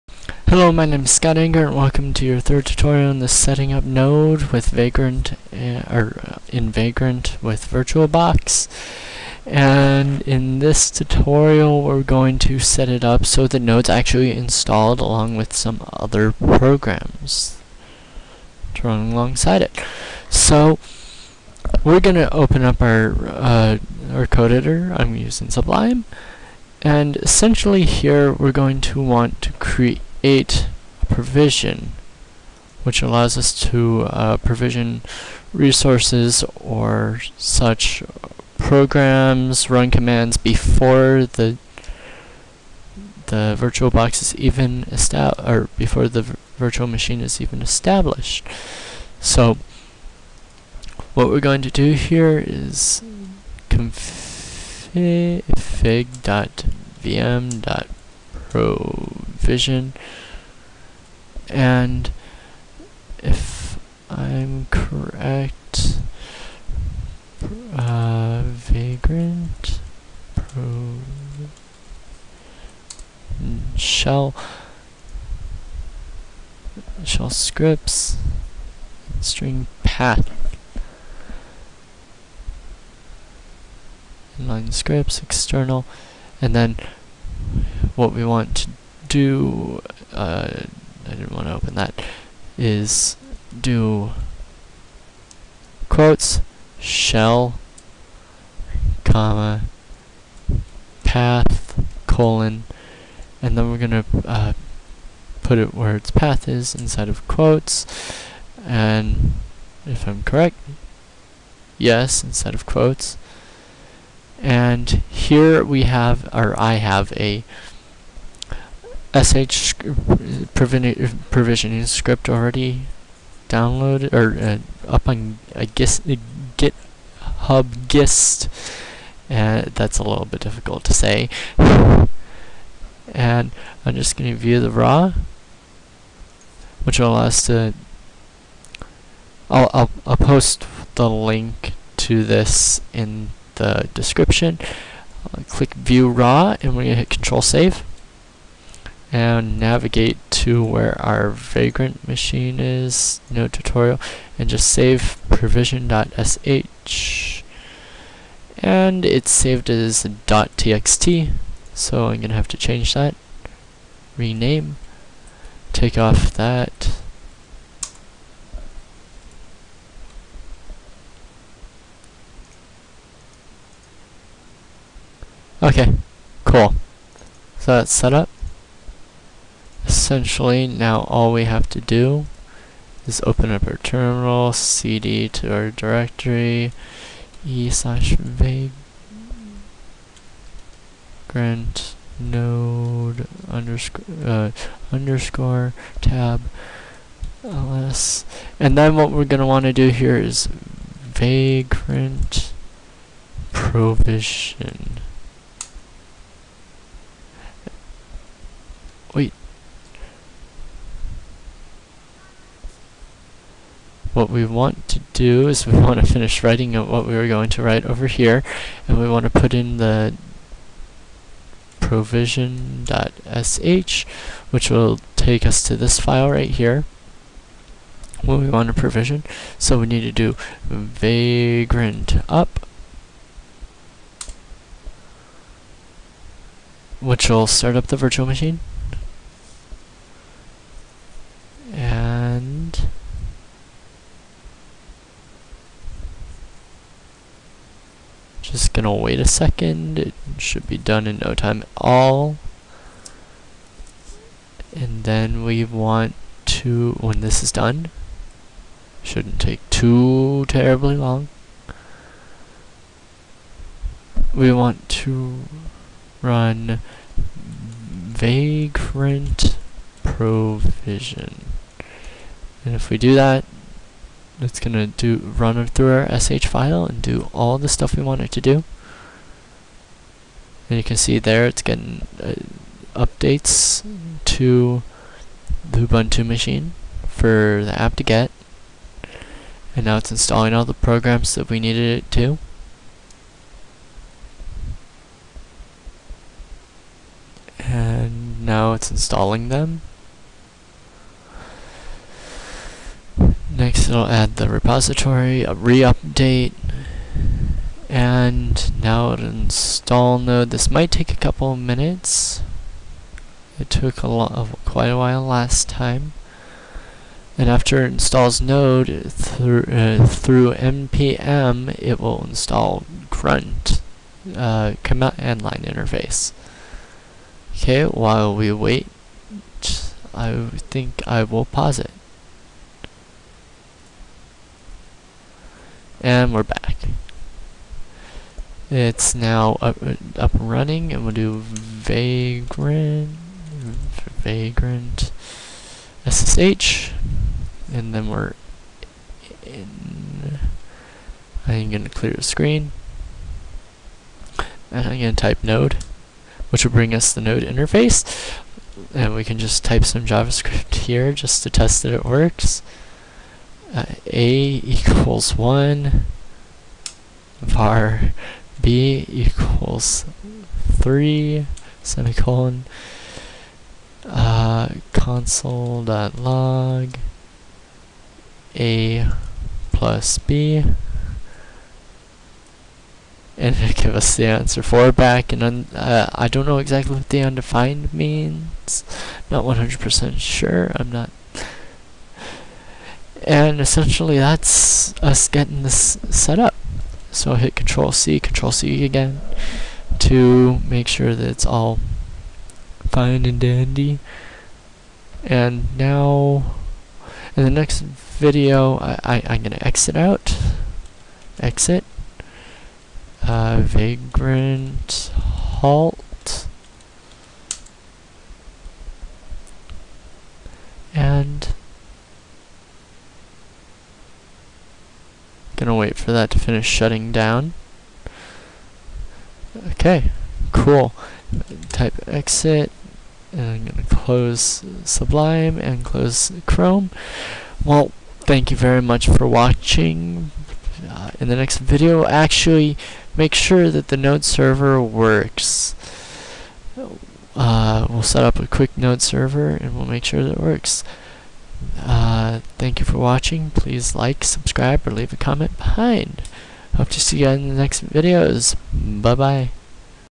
Node.js on windows - Provisioning Vagrant for Node.js
In this tutorial you will finally provision the Vagrant box to have Node.js installed. You'll also test to make that Node.js installed. In the next tutorial, you will set up a Node.js server to make sure that the Networking is working correctly.